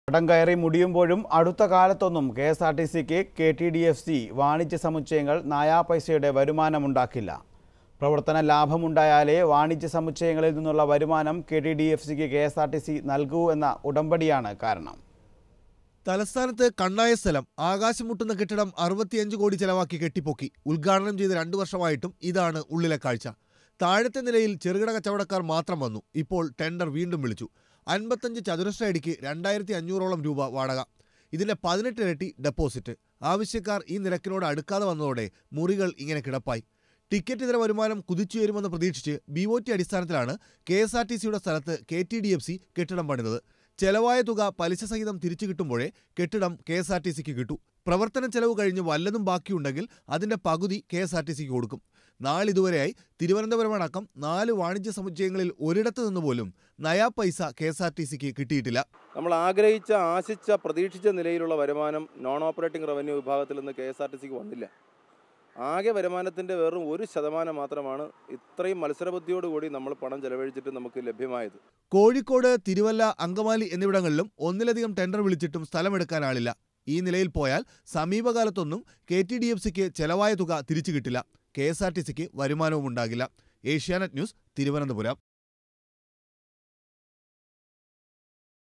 KRTC shopping complexes are no money-spinners| Asianet News Investigation
KRTC shopping complexes are no money-spinners